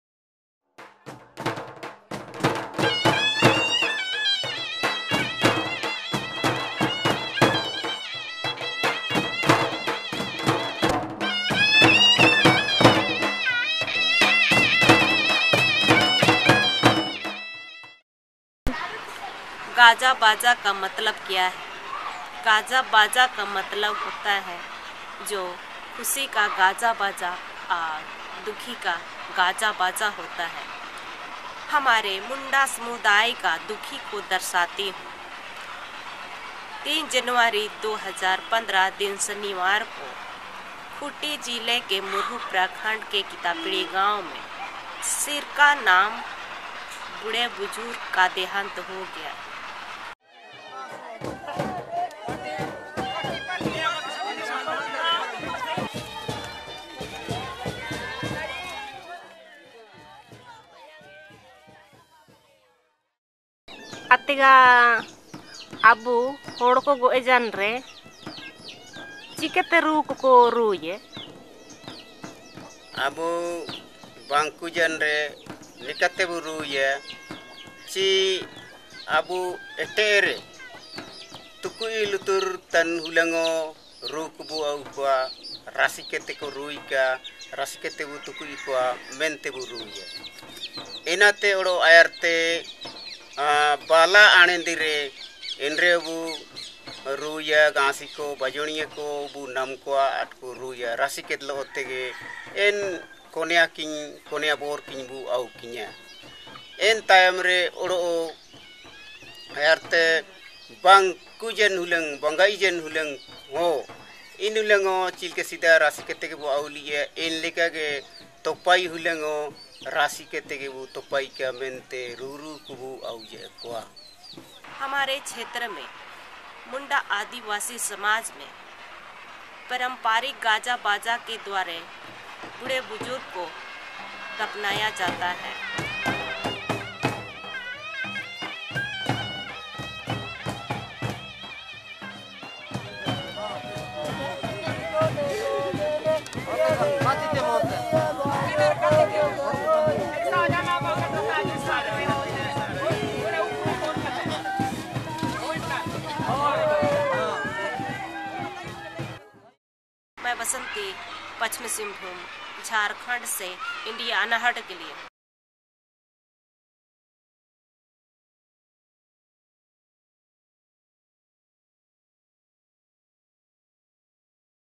Funeral procession of Munda tribe— Video Volunteer Basanti Hunni Purti Reports
This video was made by a Video Volunteers Community Correspondent Basanti Hunni Purti.